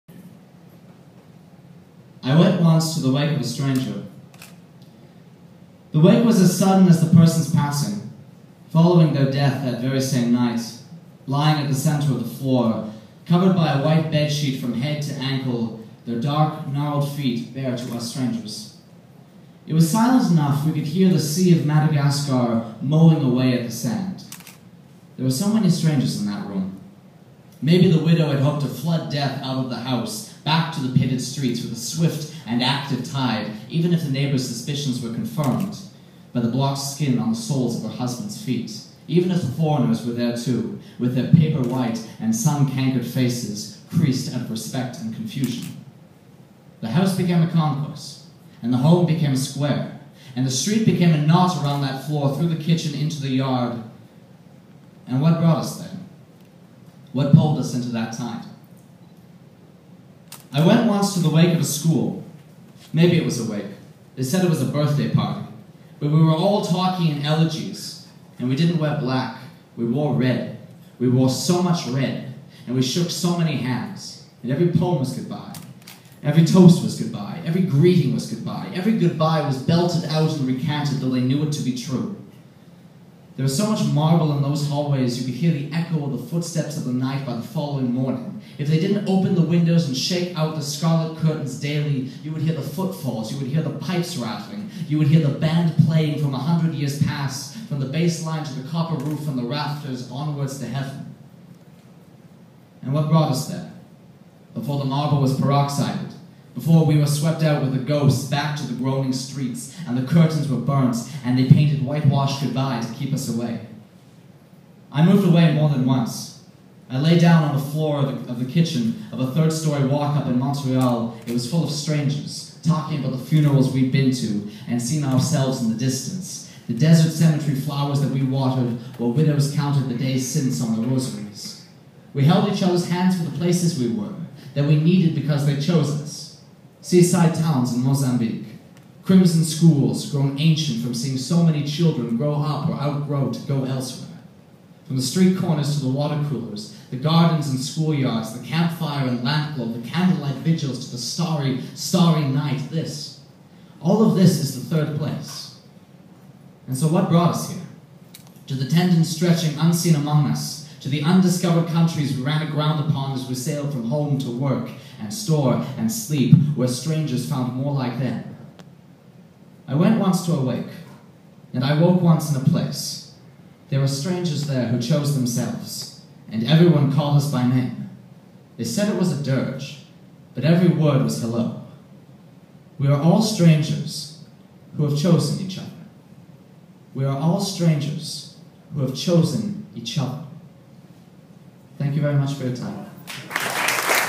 Strangers Who Choose Themselves
A spoken word performance by Simon Banderob at the Foundation's 2015 Community Celebration

Full text:

I went once to the wake of a stranger

the wake as as sudden as the person's passing
following their death quickly, that very night
lying at the centre of the floor
covered by a white bedsheet from head to ankle
their dark, gnarled feet bare to us strangers
it was silent enough we could hear the sea of Madagascar
mowing away into the sand.

So many strangers in that room
maybe the widow hoped to flood death out of the house
back to the pitted streets
with a swift tide of activity
even if the neighbours' suspicions were confirmed
by the blotched skin on the soles of her husband's feet
even if the foreigners were there too
with their paper-white and sun-cankered faces
creased out of respect and confusion.

The house became a concourse
the home became a square
the street became a knot around that floor, through that kitchen, in to the yard
the unreality of this.

What brought us here, what pulled us into the tide?

I went once to the wake of a school

maybe it was a wake, they said it was a birthday party
but we were all talking in elegies
we didn't wear black; we wore red
we wore so much red
we shook so many hands

every poem was goodbye
every toast was goodbye
every greeting was goodbye
every goodbye was belted-out and recanted through they knew it to be true.

There was so much marble in those hallways
you could hear the echo of the footsteps of the night
by the following morning
if they didn't open the windows and shake out the scarlet curtains daily
you would hear the footfalls
you would hear the pipes rattling
you would hear the band playing
from a hundred years past
from the bassline to the copper roof and the rafters
onwards to heaven.

What brought us there?
Before the marble was peroxided
and we were swept out with the ghosts
back to the groaning streets
and the curtains were burnt
and they painted whitewash goodbye to keep us away?

I moved away more than once
I lay down on the kitchen floor
of a third-story-walkup in Montreal
it was full of strangers
talking about the funerals we'd been to
and seen ourselves in the distance
the desert cemetery flowers we watered
while widows counted the days since on their rosaries.

We held each others' hands for the places we were
that we needed because they chose us
seaside towns in Mozambique
crimson schools become ancient from seeing so many children
grow up or outgrow
to go elsewhere.

From the street corners
to the water coolers
the gardens and schoolyards
campfire and lampglow
candle light vigils to the starry, starry nights
this, all of this, is the third place.

What brought us here?
To the tendons stretching unseen among us
to the undiscovered countries we ran aground upon
as we sailed between home and work and store and sleep
where strangers found more like them?

I went once to a wake
I woke once in a place
there were strangers there who chose themselves
and everyone called us by name
they said it was a dirge
but every word was hello.

We are all strangers
who chose each other.